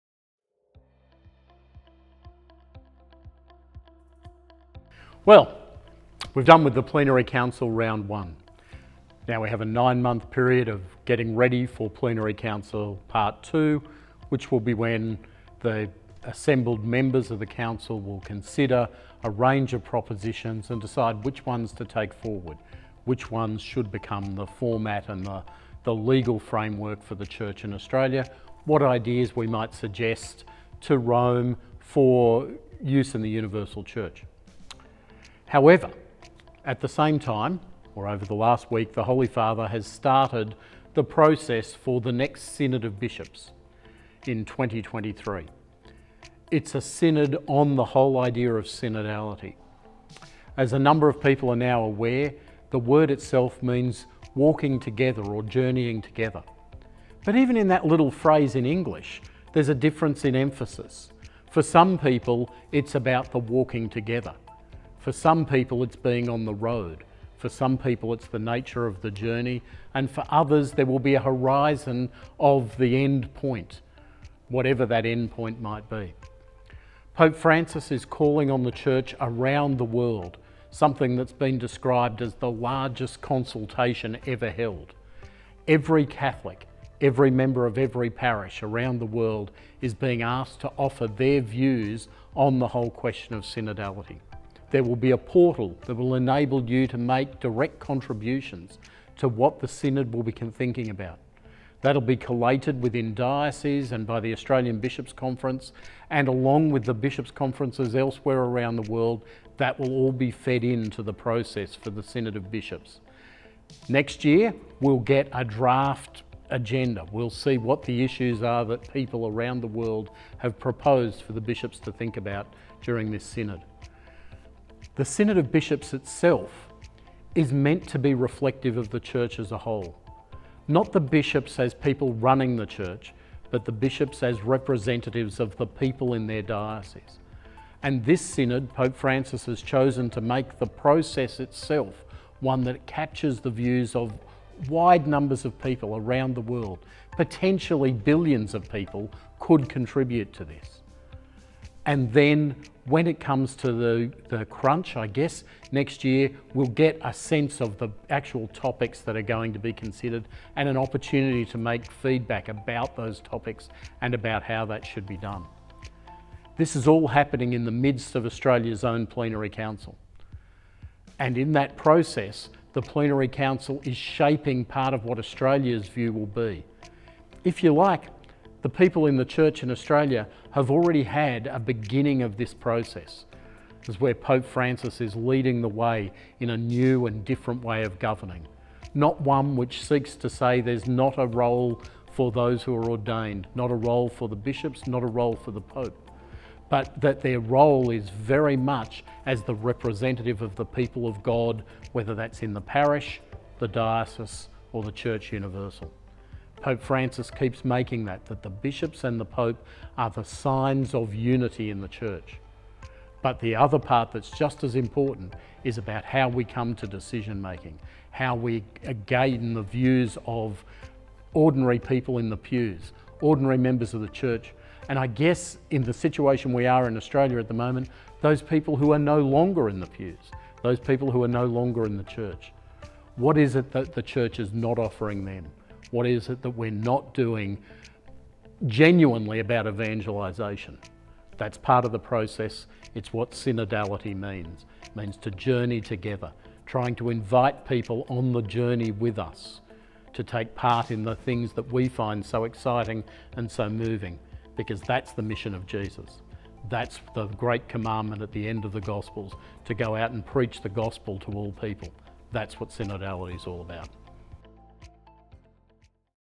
Synod on Synodality - Patrick McArdle explains
Patrick McArdle explains the upcoming international Synod on Synodality